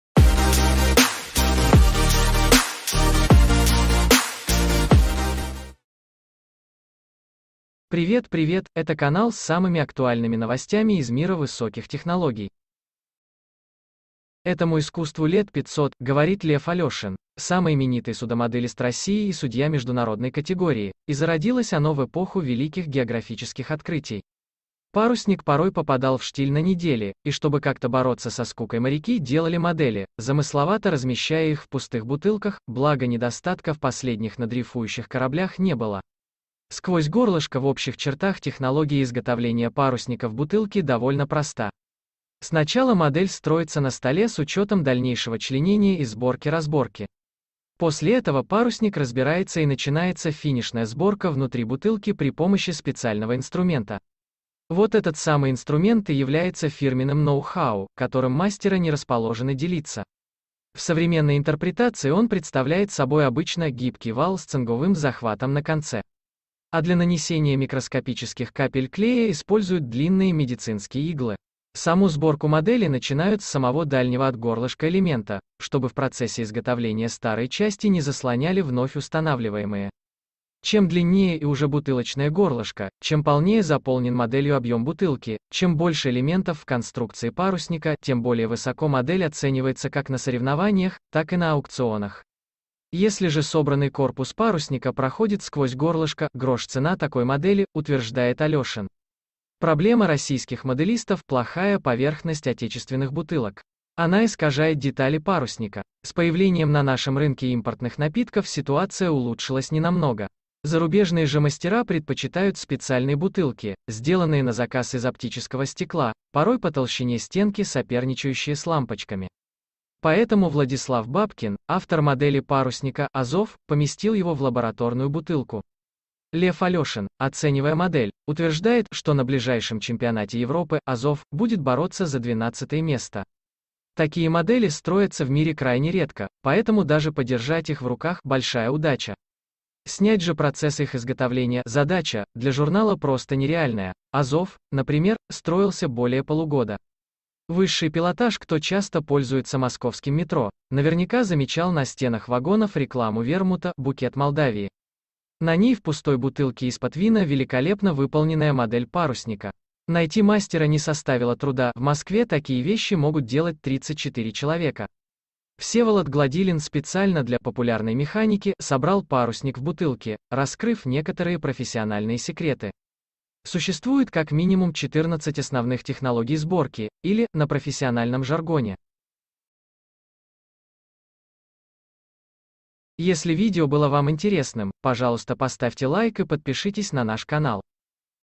Запузыривание: корабли в бутылках
Как попадает модель парусника в бутылку? Одна из версий – мастера аккуратно отпиливают донышко, а потом, поместив игрушку внутрь, приклеивают его обратно. На самом деле все гораздо интереснее.